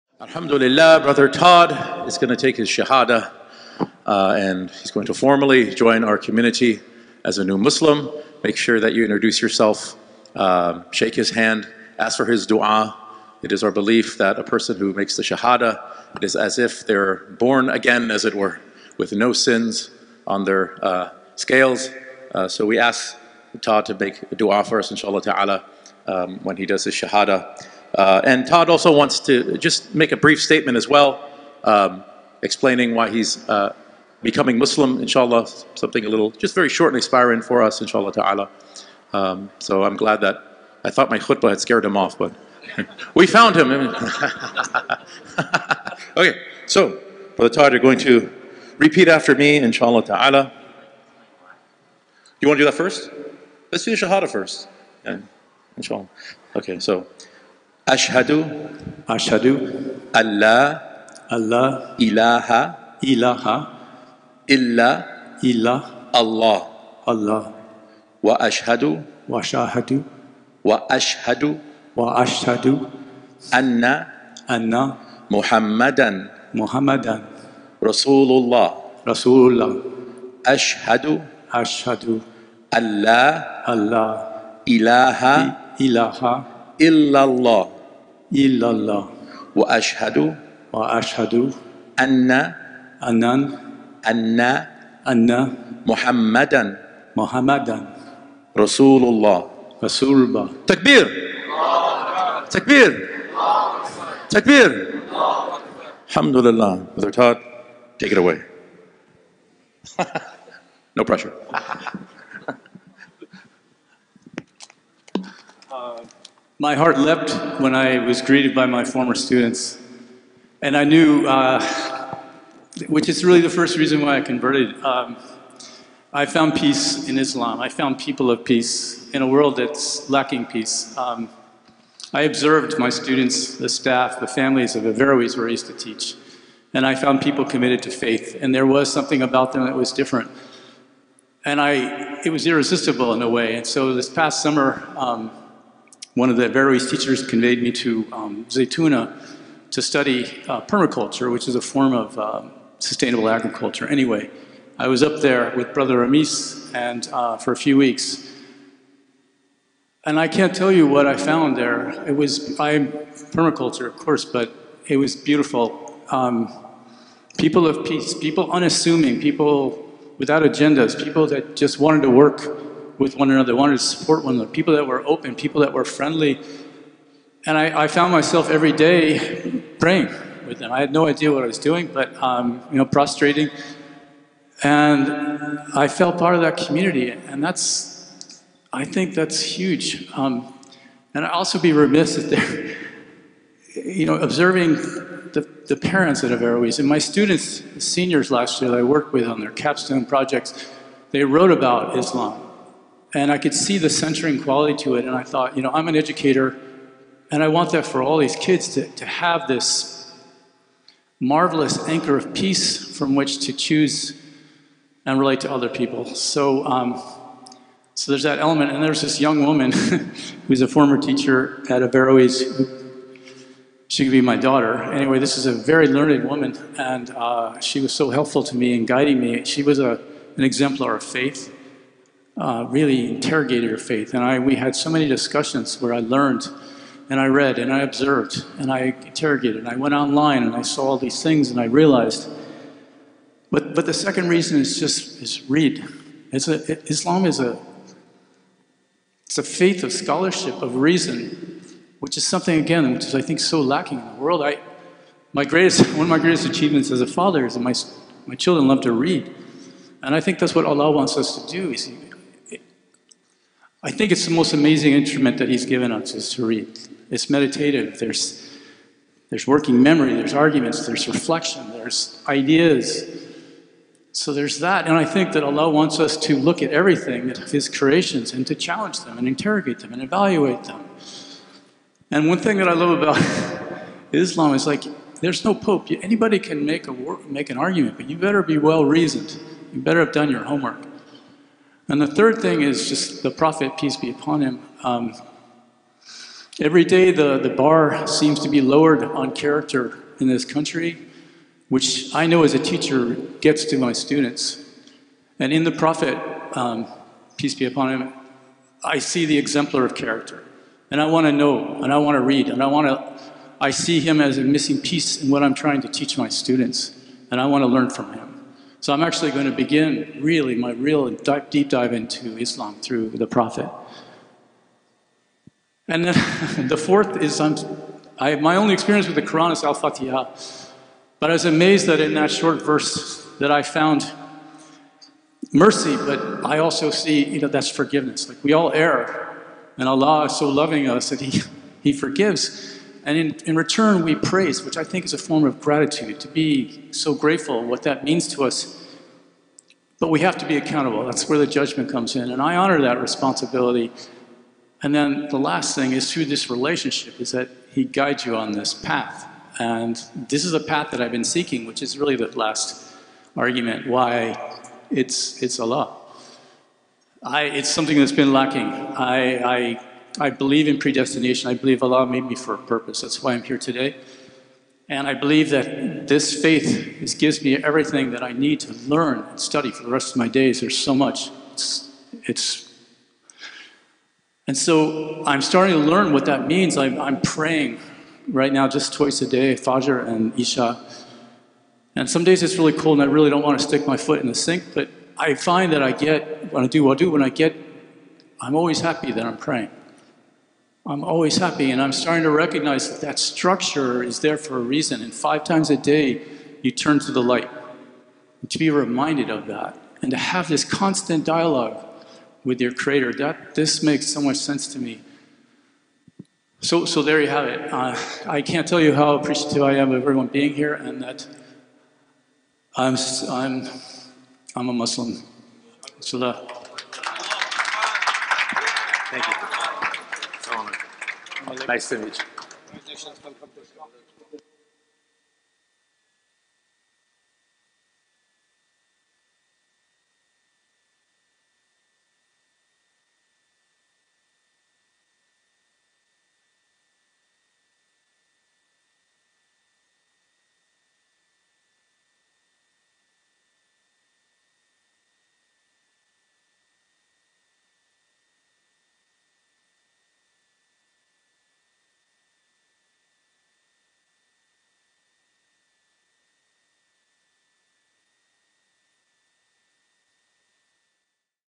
Todd's Shahadah (Declaration of Faith) | Dr. Ali Ataie
Brother Todd Armstrong takes his Shahadah (Declaration of Faith) with Ustadh Dr. Ali Ataie after his Jumu'ah Khutbah

Br. Todd is a former teacher at Averroes High School in Hayward, California. Inspired by his Muslim students and a permaculture course at Zaytuna College, Sidi Armstrong began his spiritual journey towards finding Islam.

This Shahada took place on June 7, 2024 at the Muslim Community Center - East Bay (MCC East Bay) in Pleasanton, California.

If you are a new Muslim in the San Francisco Bay Area who would like to receive updates from MCC for new Muslims, please